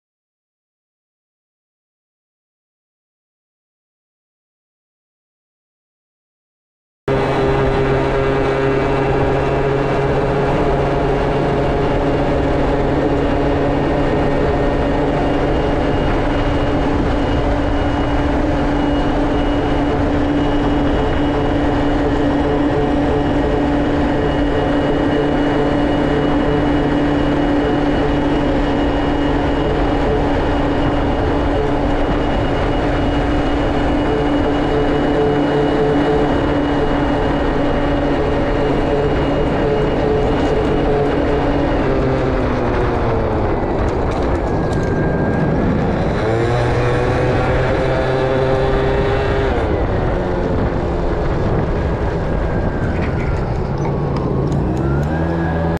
ヤマハパッソル改パッジョグで走ってみた／ピットへ給油に向かう
念のために給油しようといつものＧＳへ向かう